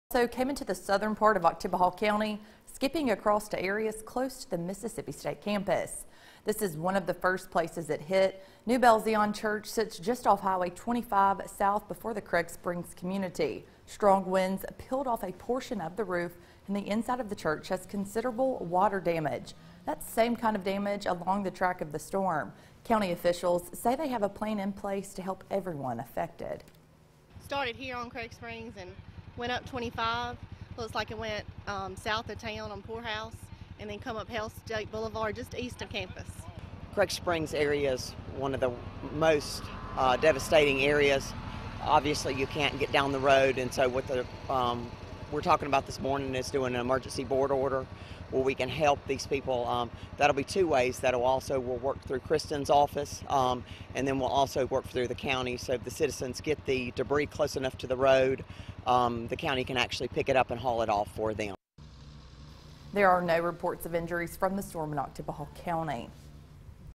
OKT RESPONSE
The storm also came into the southern part of Oktibbeha County, skipping across to areas close to the Mississippi State campus.